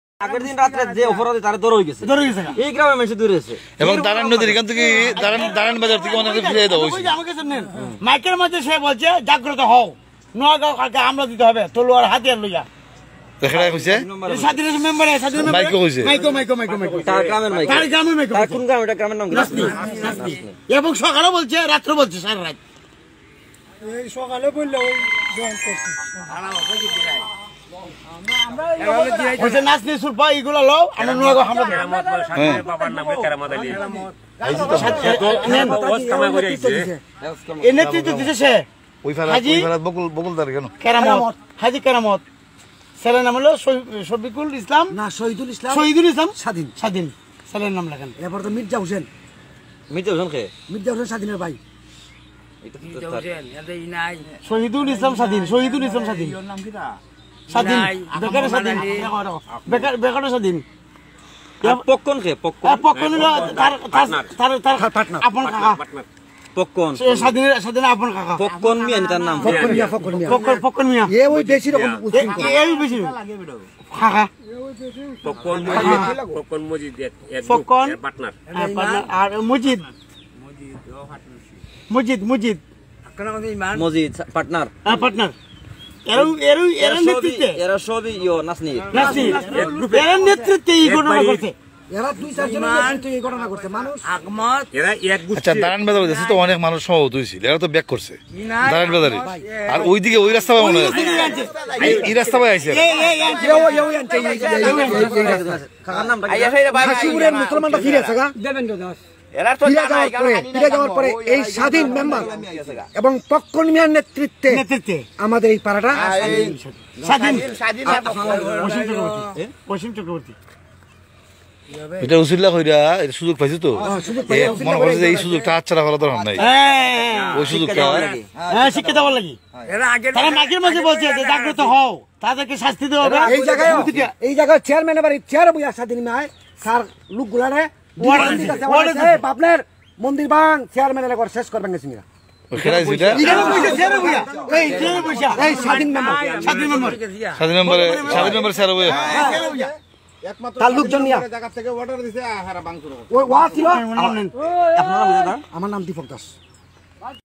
শাল্লায় হিন্দু গ্রামে হামলা নিয়ে যা বলল এই হিন্দু ভাই দেখুন (ভিডিও) | news world
শাল্লায় হিন্দু গ্রামে হামলা নিয়ে মুখ খুলল ঐ গ্রামের এক হিন্দু ভাই, হামলা করে যুবলীগ নেতা স্বাধীন।
দেখুন বিস্তারিত

(ভিডিওটি শেয়ার করে ছড়িয়ে দিন)

@voice of tawheed

news world

ঘর-সংসার দিল্লি, সরকারি চাকরি বাংলাদেশে

ভিডিওটি শেয়ার করে সবাইকে দেখার সুযোগ করে দিন.